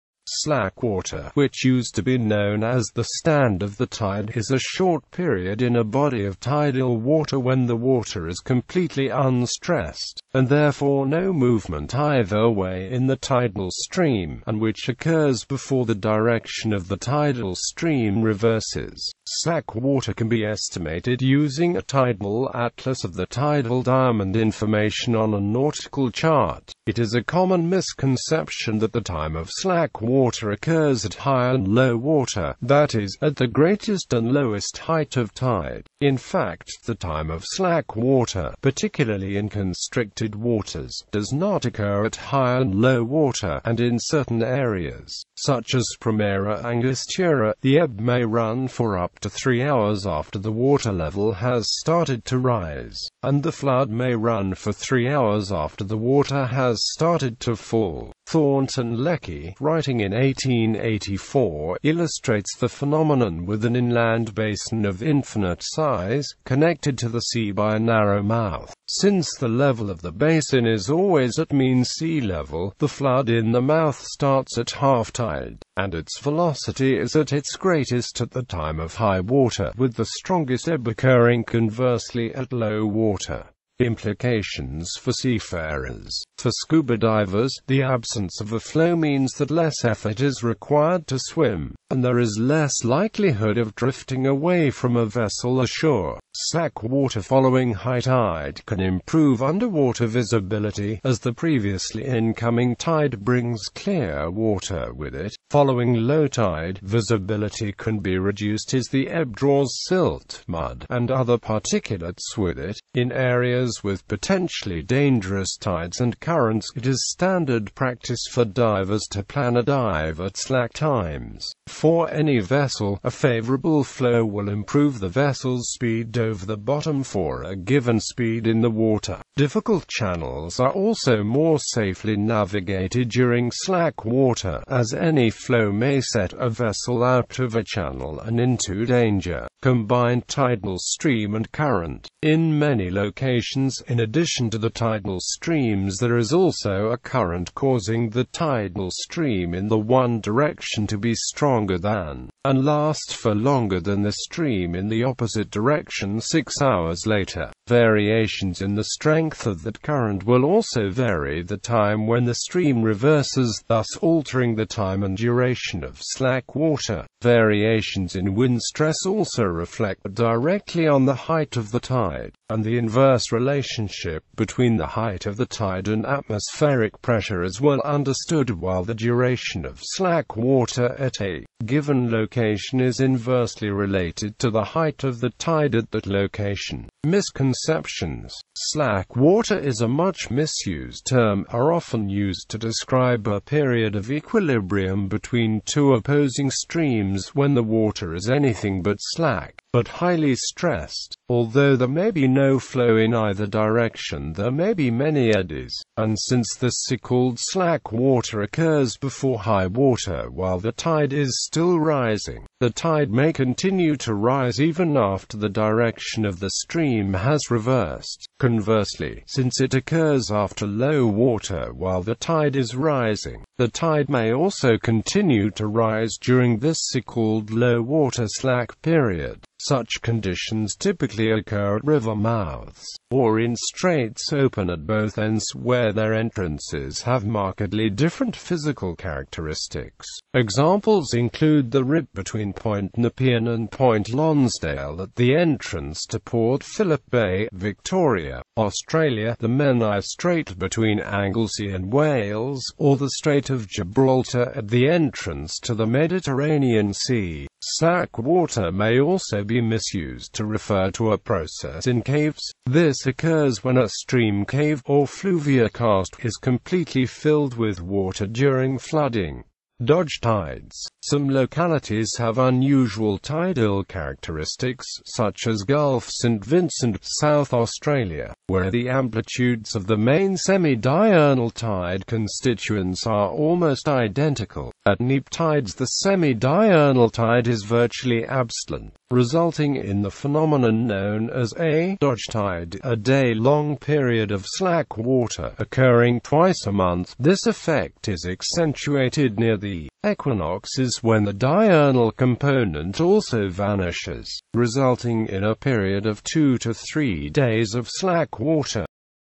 Slack water
Ad-free videos.

Slack water, which used to be known as 'the stand of the tide', is a short period in a body of tidal water when the water is completely unstressed, and therefore no movement either way in the tidal stream, and which occurs before the direction of the tidal stream reverses.Slack water can be estimated using a tidal atlas or the tidal diamond information on a nautical chart.It is a common misconception that the time of slack water occurs at high and low water, that is, at the greatest and lowest height of tide.In fact, the time of slack water, particularly in constricted waters, does not occur at high and low water, and in certain areas, such as Primera Angostura, the ebb may run for up to three hours after the water level has started to rise, and the flood may run for three hours after the water has started to fall.

---Image-Copyright-and-Permission---
About the author(s): Lichtspiel
---Image-Copyright-and-Permission---